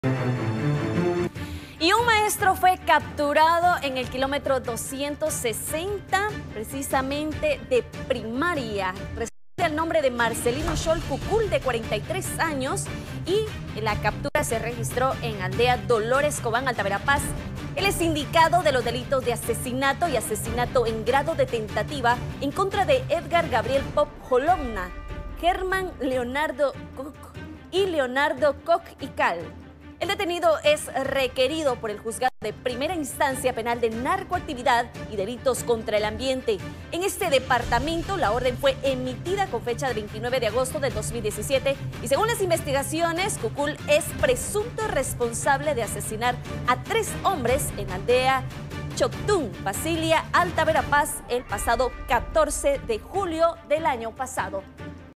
Capturan a maestro de primaria
En Alta Verapaz, agentes de la PNC capturaron a un maestro de primaria acusado de asesinato.